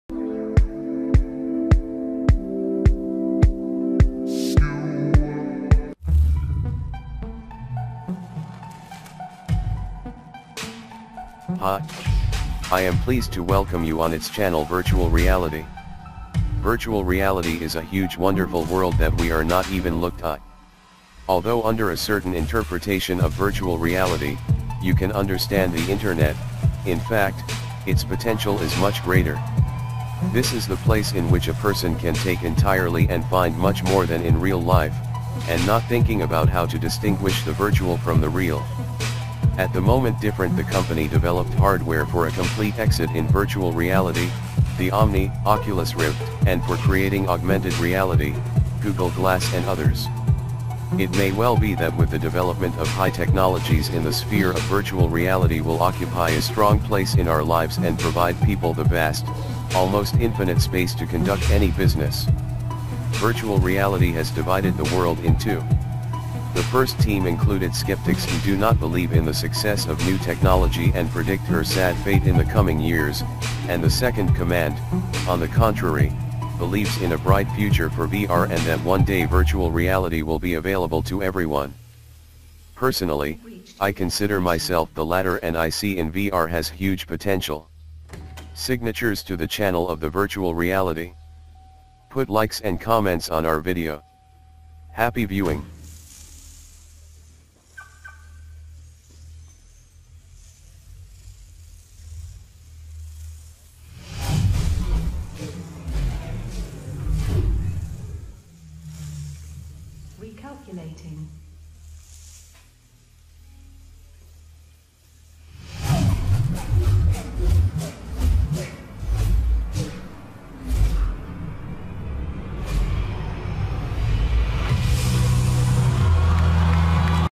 Channel trailer - Virtual Reality
Hi ! I am pleased to welcome you on its channel VIRTUAL REALITY .
Virtual reality is a huge wonderful world that we are not even looked eye.
 Although under a certain interpretation of virtual reality, you can understand the Internet, in fact, its potential is much greater.
This is the place in which a person can take entirely and find much more than in real life, and not thinking about how to distinguish the virtual from the real.
At the moment different the company developed hardware for a complete exit in virtual reality: the Omni, Oculus Rift, and for creating augmented reality: Google Glass and others.
 It may well be that with the development of high technologies in the sphere of virtual reality will occupy a strong place in our lives and provide people the vast, almost infinite space to conduct any business.
Virtual reality has divided the world in two.
The first team included skeptics who do not believe in the success of new technology and predict her sad fate in the coming years, and the second command, on the contrary, believes in a bright future for VR and that one day virtual reality will be available to everyone.
Personally, I consider myself the latter and I see in VR has huge potential.
Signatures to the channel of the virtual reality !
Put likes and comments on our video.
Happy viewing!